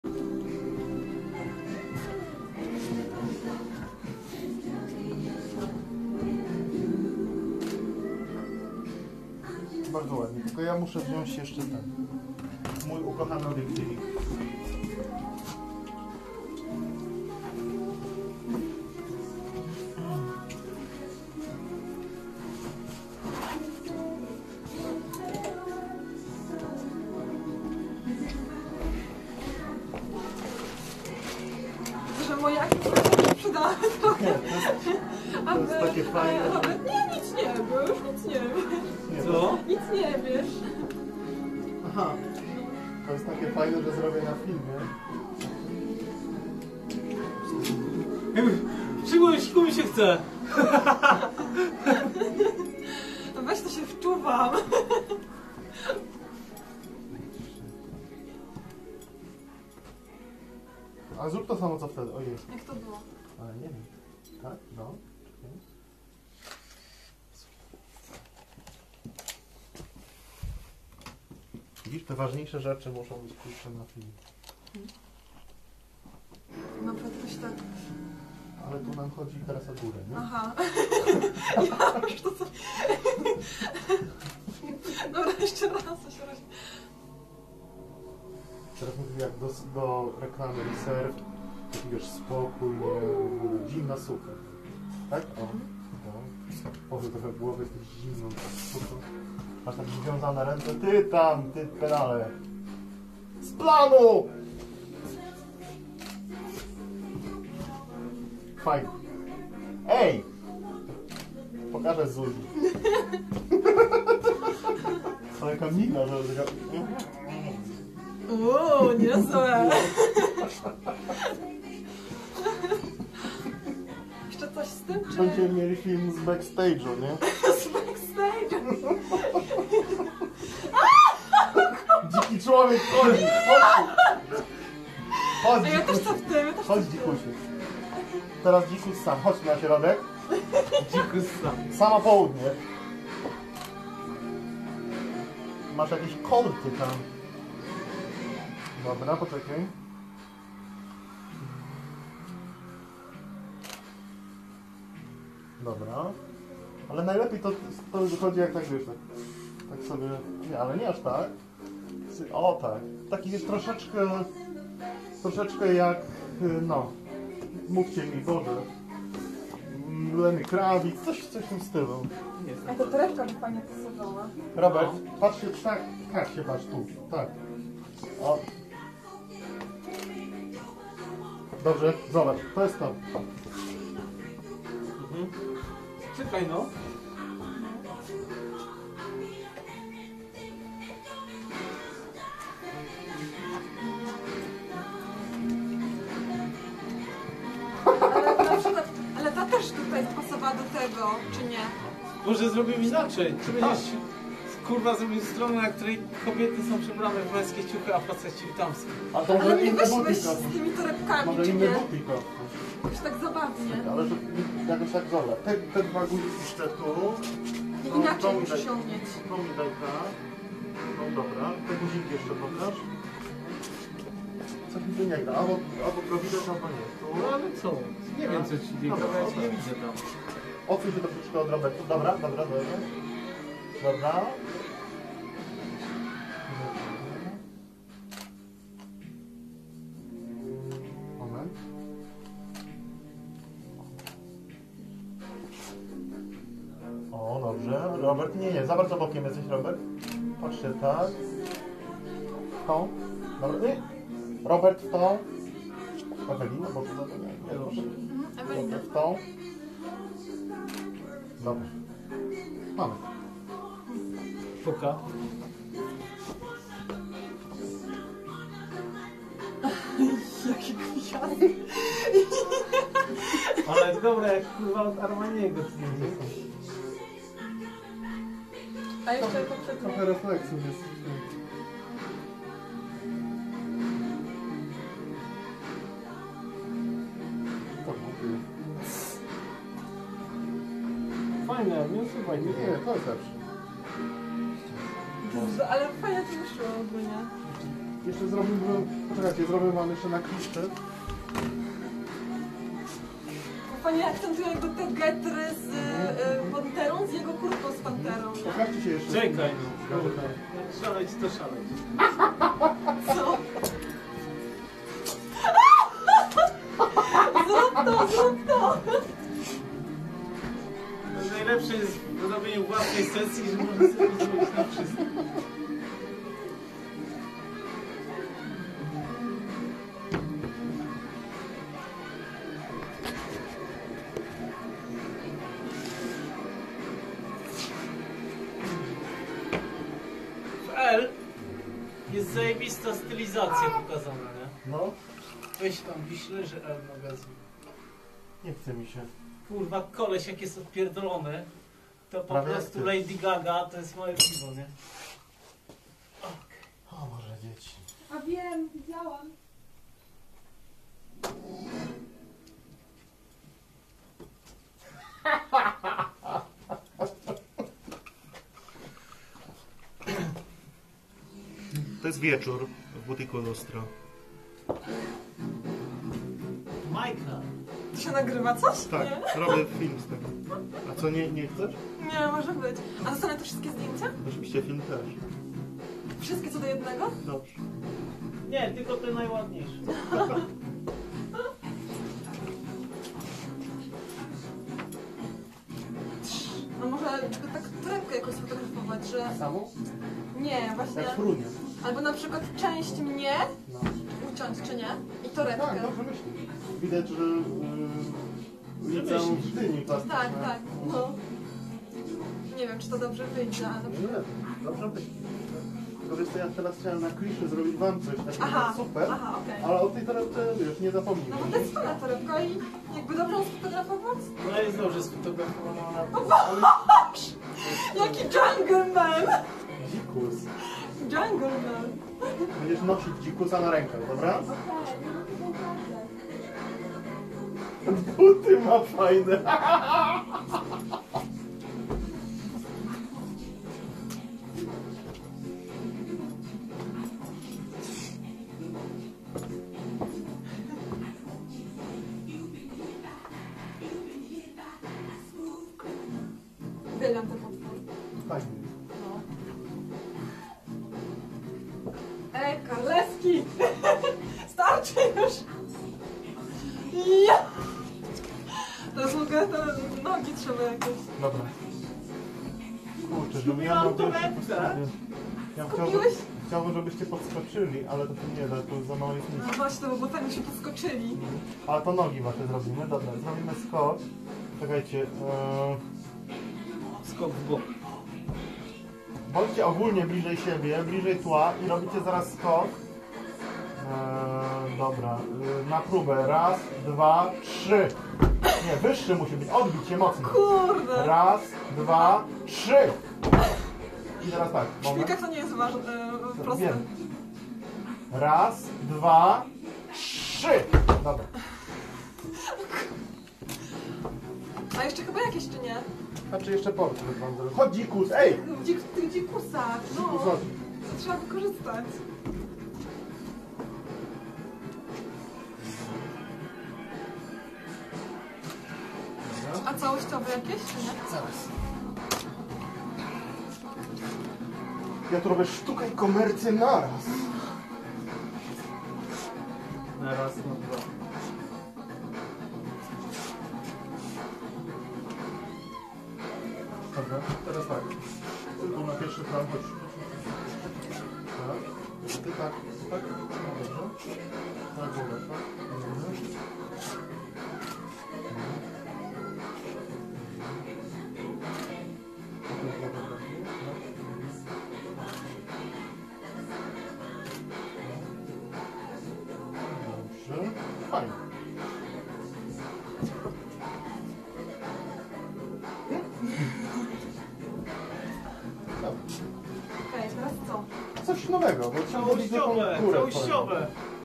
Butik Luztro.MP4
BUTIK LUZTRO sesja 9 luty 2010 modelka Ewelina, fotograf Jacek Chmielewski